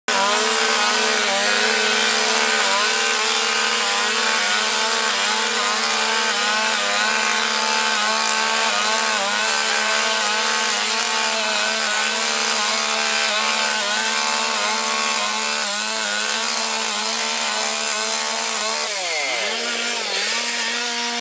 Zawody drwali Bobrowa 2014 - Roman Szala
XV Międzynarodowe Zawody Drwali w Bobrowie. Okrzesywanie pnia - Roman Szala (Jełowa). Więcej zdjęć i artykuł na